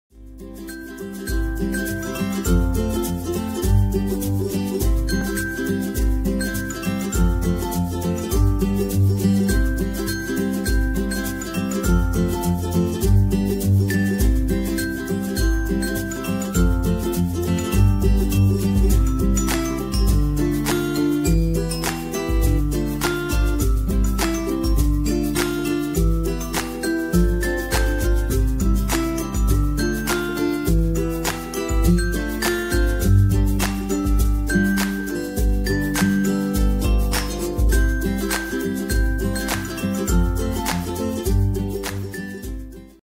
Photo Video Slideshow - Promo Baby's First Birthday Theme Sample
This sample shows photo video slideshow created using baby's birthday party pictures. to create a similar video for your baby's birthday using Promo Baby's First Birthday Theme with music.